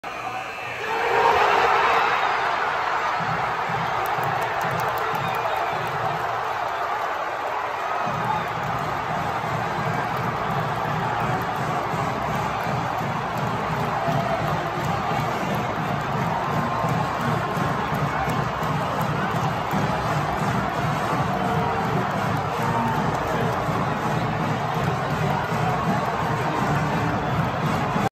Indiana students storm court after IU defeats Purdue
Indiana students storm court after IU Hoosiers defeat Purdue at Assembly Hall 68-65 January 20, 2022.